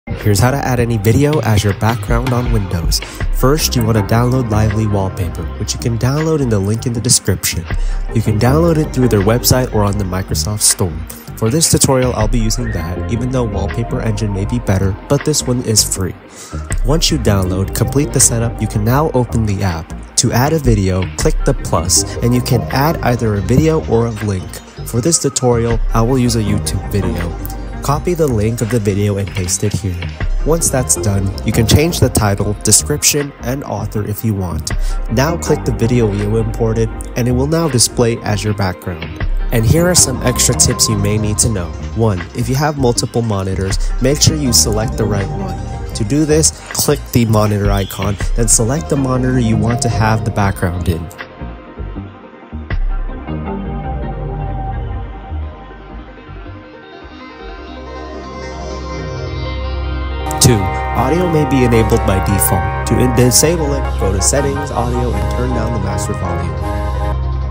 How to add any video as your background on Windows | QuickTips
-- Links --
Lively Wallpaper: